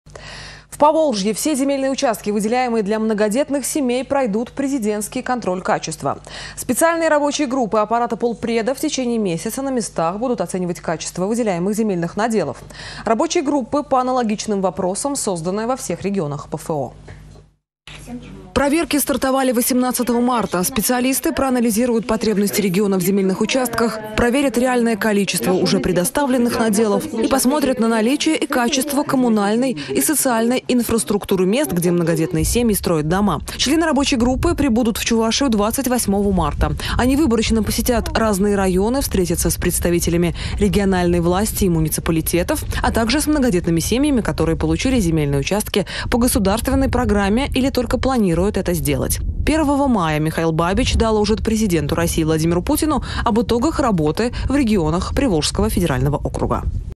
Президентский контроль качества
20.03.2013
Земельные участки для многодетных семей пройдут президентский контроль качества. Специальные рабочие группы аппарата полпреда в течение месяца на местах будут оценивать качество выделяемых земельных наделов. Рабочие группы по аналогичным вопросам созданы во всех регионах ПФО.